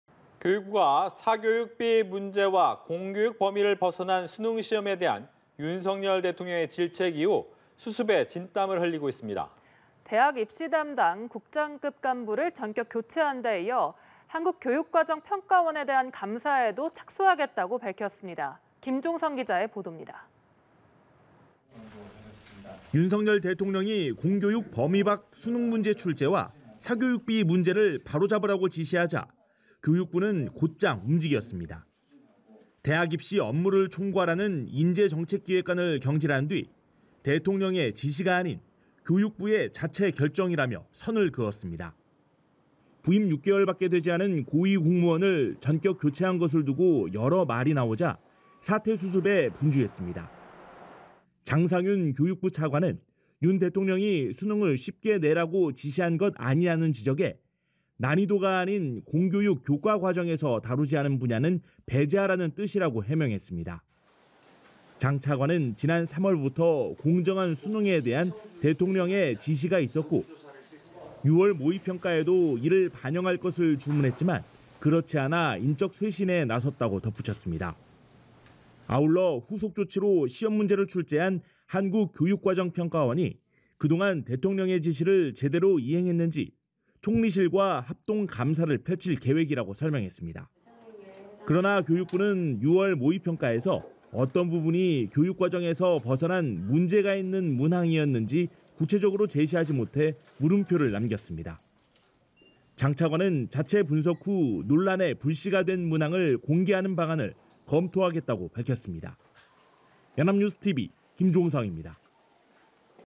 문책 이어 감사…불호령 맞은 교육부, 수습 '진땀' / 연합뉴스TV (YonhapnewsTV)
문책 이어 감사…불호령 맞은 교육부, 수습 '진땀'

[앵커]

교육부가 사교육비 문제와 공교육 범위를 벗어난 수능시험에 대한 윤석열 대통령의 질책 이후 수습에 진땀을 흘리고 있습니다.

대학 입시 담당 국장급 간부를 전격 교체한 데 이어 한국교육과정평가원에 대한 감사에도 착수하겠다고 밝혔습니다.

김종성 기자입니다.

[기자]

윤석열 대통령이 공교육 범위 밖 수능 문제 출제와 사교육비 문제를 바로 잡으라고 지시하자 교육부는 곧장 움직였습니다.

대학 입시 업무를 총괄하는 인재정책기획관을 경질한 뒤, 대통령의 지시가 아닌 교육부의 자체 결정이라며 선을 그었습니다.

부임 6개월 밖에 되지 않은 고위 공무원을 전격 교체한 것을 두고 여러 말이 나오자 사태 수습에 분주했습니다.

장상윤 교육부 차관은 윤 대통령이 수능을 쉽게 내라고 지시한 것 아니냐는 지적에 "난이도가 아닌 공교육 교과과정에서 다루지 않은 분야는 배제하라는 뜻"이라고 해명했습니다.

장 차관은 지난 3월부터 '공정한 수능'에 대한 대통령의 지시가 있었고, 6월 모의평가에도 이를 반영할 것을 주문했지만 그렇지 않아 인적 쇄신에 나섰다고 덧붙였습니다.

아울러 후속 조치로 시험문제를 출제한 한국교육과정평가원이 그동안 대통령의 지시를 제대로 이행했는지 총리실과 합동 감사를 펼칠 계획이라고 설명했습니다.

그러나 교육부는 6월 모의평가에서 어떤 부분이 교육과정에서 벗어난 '문제가 있는 문항'이었는지 구체적으로 제시하지 못해 물음표를 남겼습니다.

장 차관은 자체 분석 후 논란의 불씨가 된 문항을 공개하는 방안을 검토하겠다고 말했습니다.